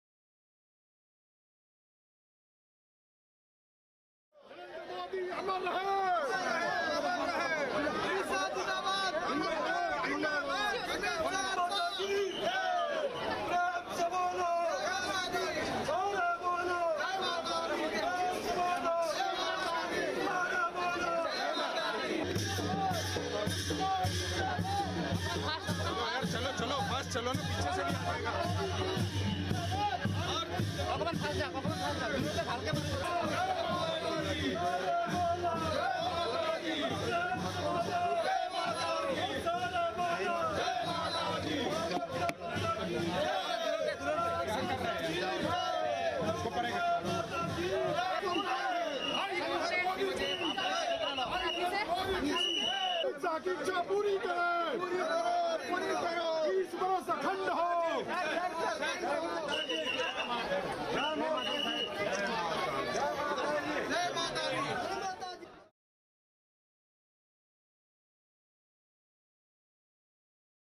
Guwahati के Kamakhya मंदिर में Amit Shah ने की पूजा-अर्चना
Bharatiya Janata Party (BJP) president Amit Shah on Wednesday prayed at the Kamakhya Temple on the occasion of Durga Ashtami.